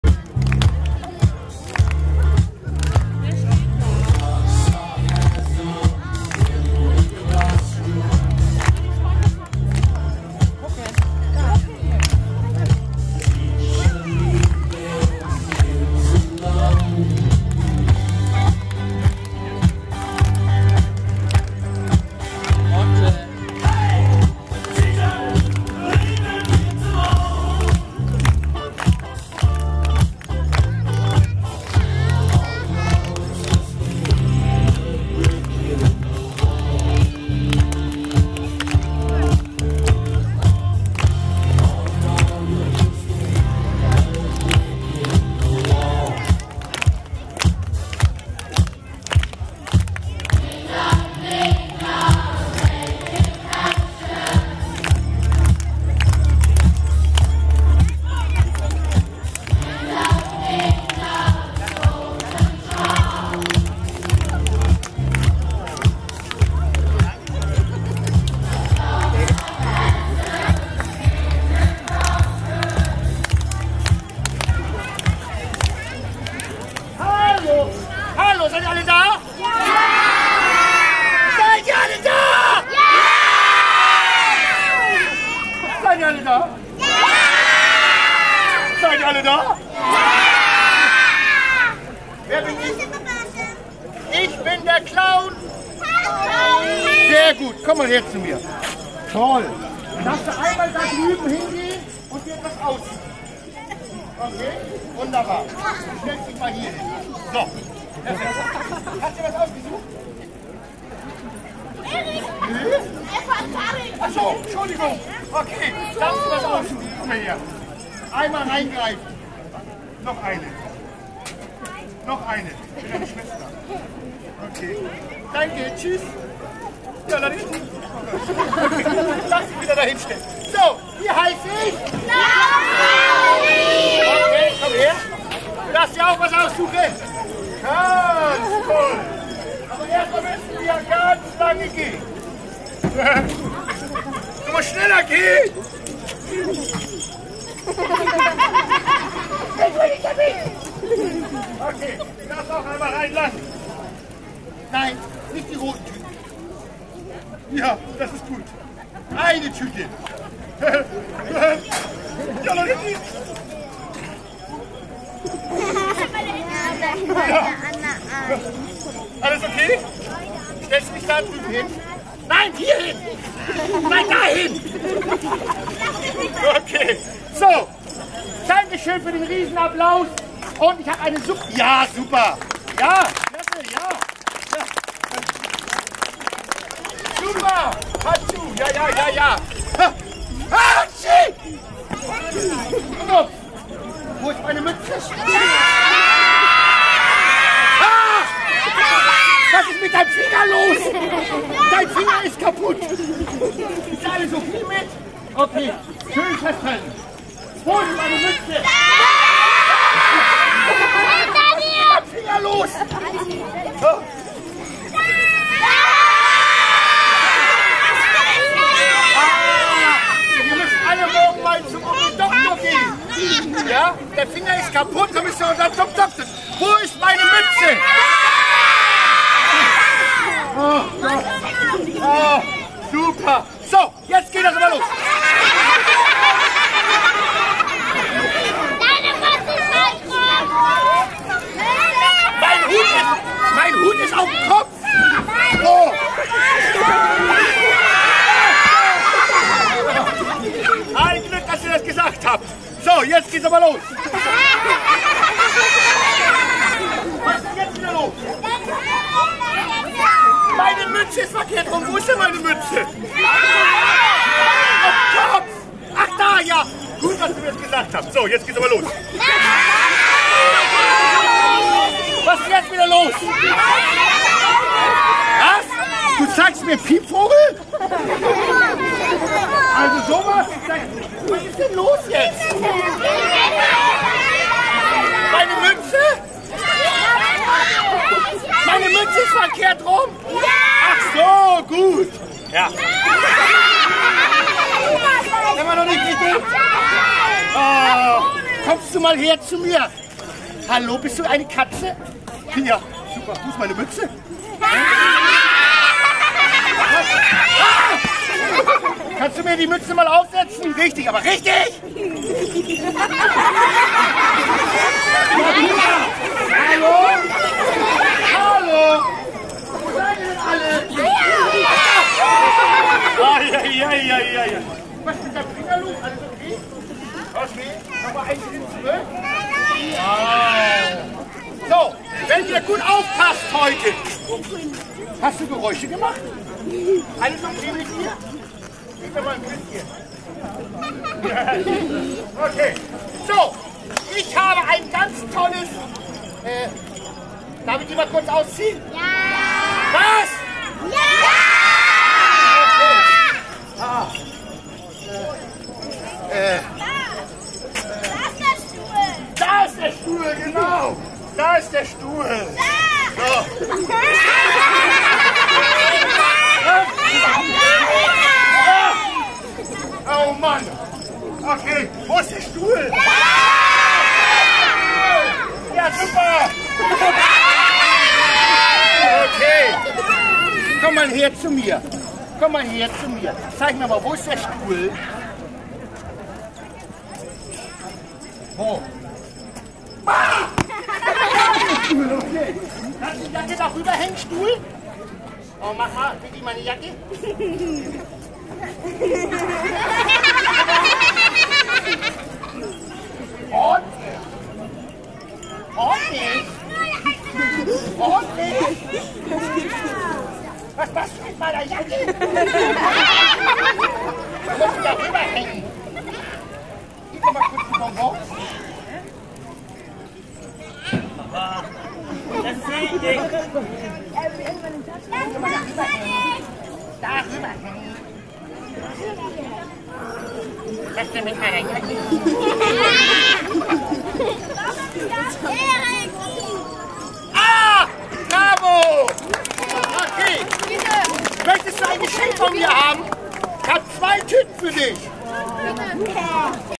Clown Pauli in Dersau 21.07.2012 Feuerwehrfest
Feuerwehrfest Dersau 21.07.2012 CLOWN PAULI